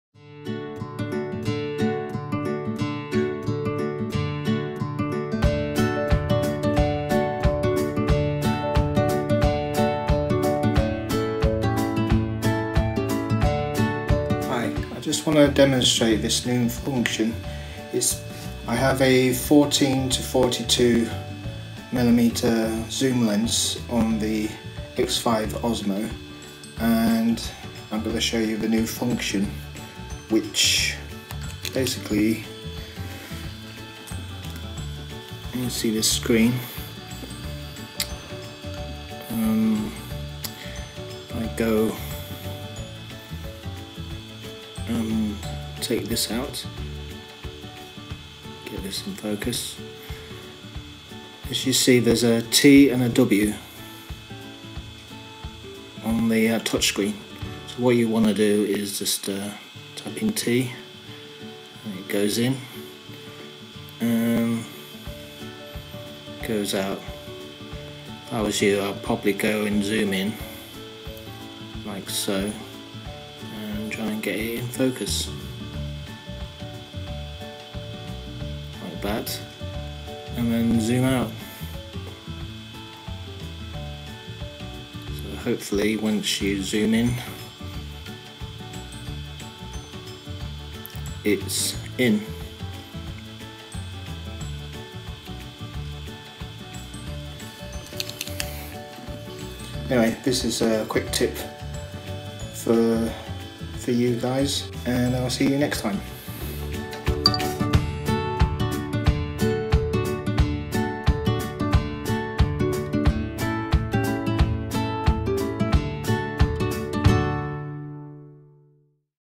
DJI Osmo, Using Zoom With The X5 DJI Osmo Along With New DJI Go 2.8.4App
DJI Osmo, Using Zoom With The X5 DJI Osmo Along With New DJI Go App version 2.8.4 this requires a 14-42mm zoom lens to alter the focal length...